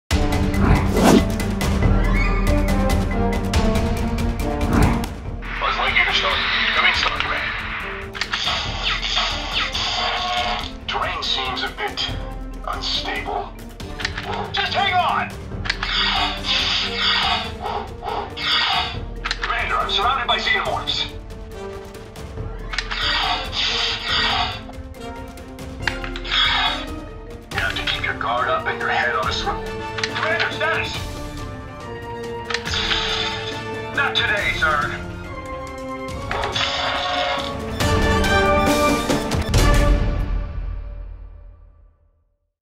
Lightyear: Подвижная фигурка Базз Лайтер 30см со звуком HHJ760
Откройте для себя Disney Pixar Lightyear. Оказавшись на неизведанной планете, Базз Лайтер и его товарищи по команде должны будут сразиться с Зургом. Большая говорящая фигурка Базз Лайтер оснащена звуковыми и световыми эффектами. Благодаря чему можно ощутить реалистичные действия как из фильма.